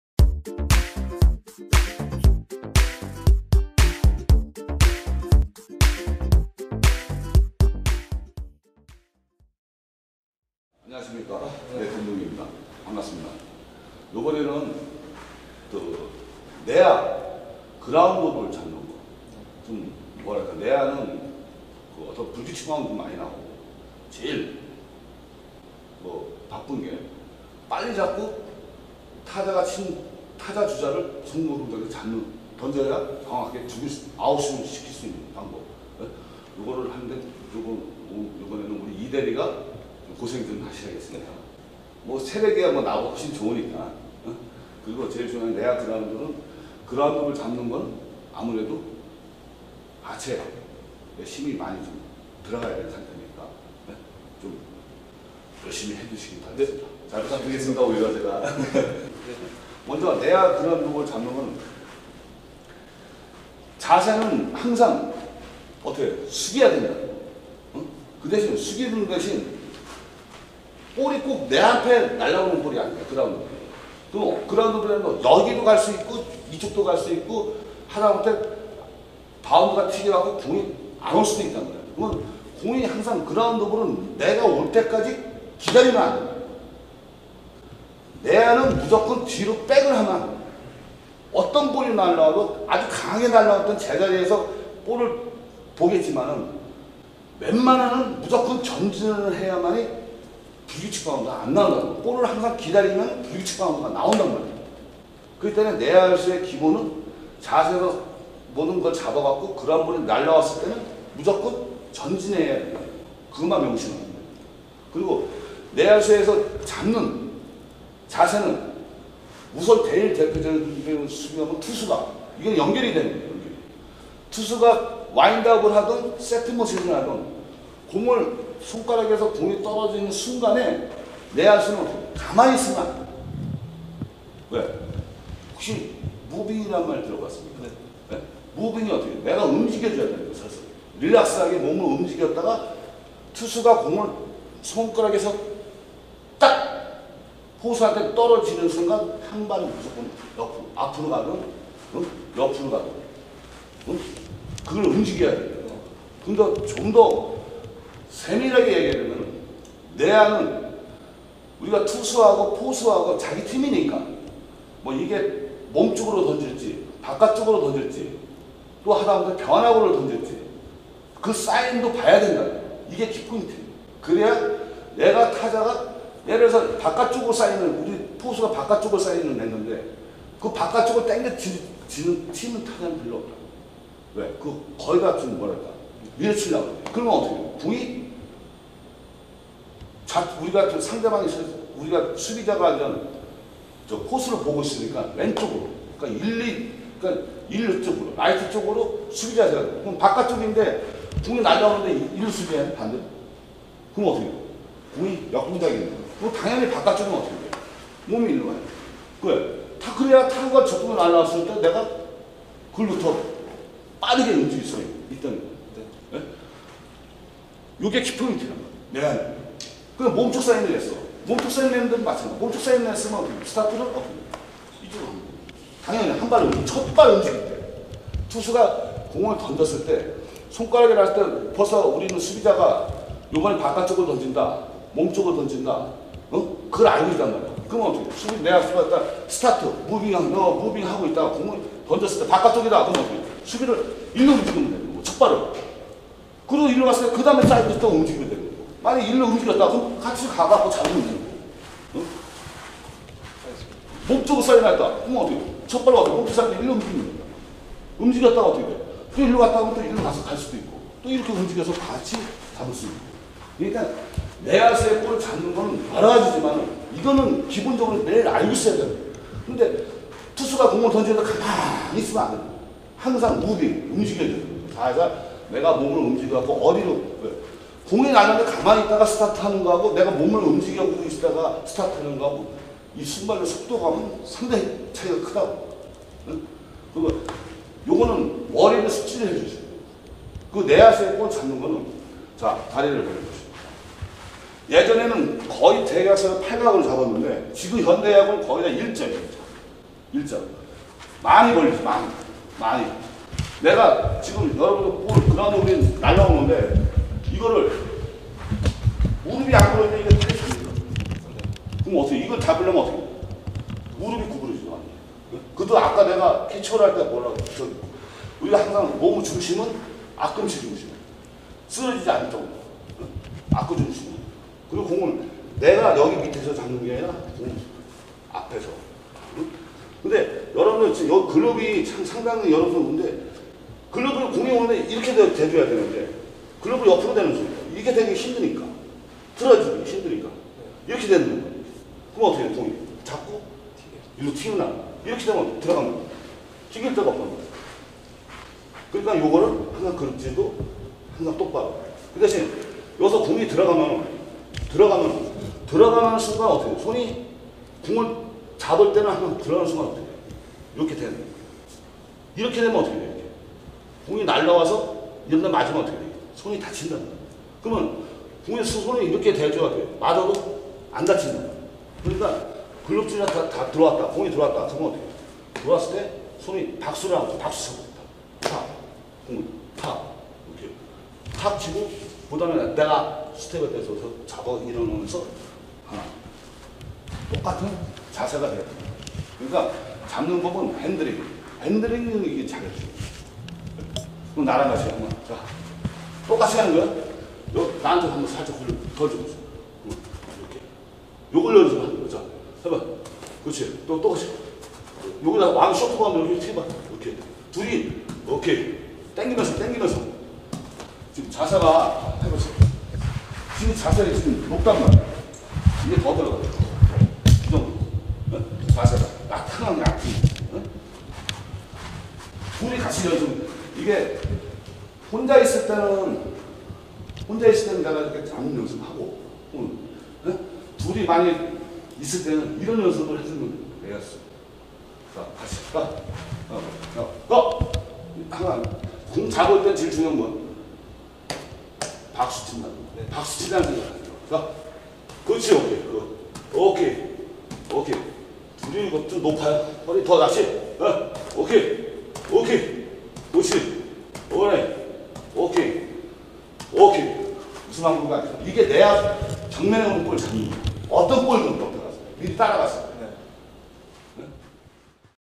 RSB 스크린 야구와 함께하는 김동기의 원포인트 레슨 #03 내야 수비-그라운드 볼 잡기
스크린야구는 당연 RSB!!! 원하는 직구 스피드와 변화구를 랜덤으로 타격함은 물론 실전 피칭과 타격 시물레이터로 타구분석까지. 이것이 진정 스크린야구다!!!